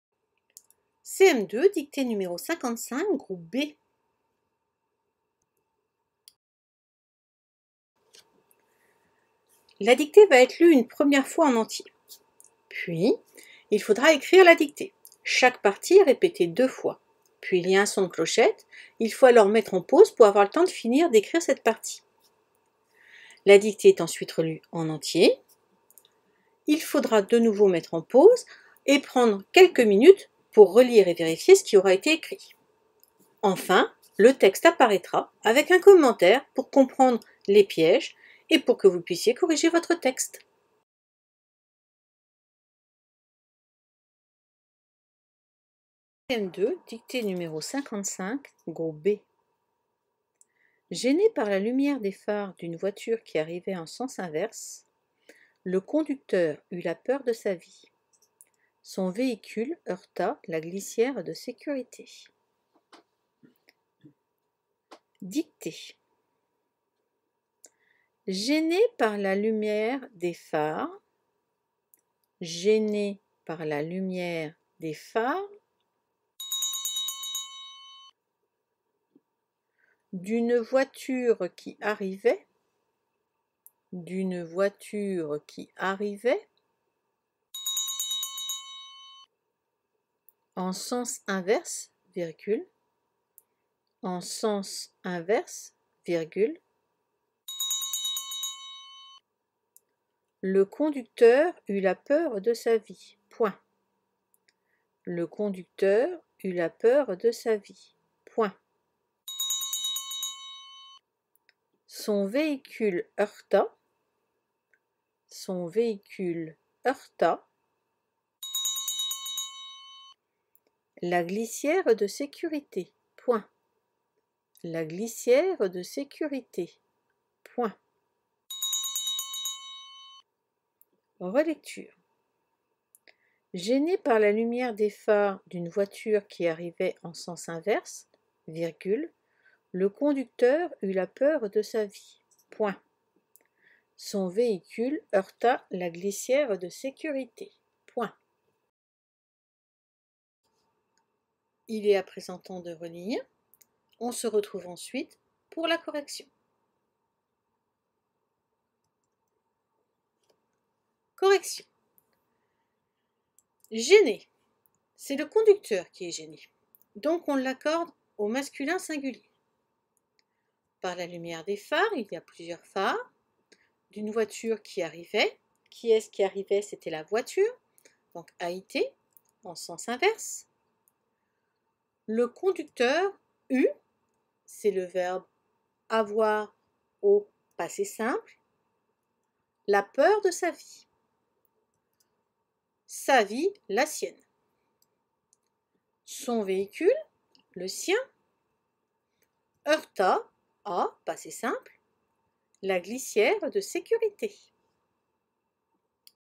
CM2 : dictée non préparée n°55, groupe B
Notions travaillées :
- le passé simple
- homophones grammaticaux

Chaque texte est disponible en 3 versions:
groupe A: le texte en entier
groupe B: un texte plus court
groupe C: un texte raccourci dicté beaucoup plus lentement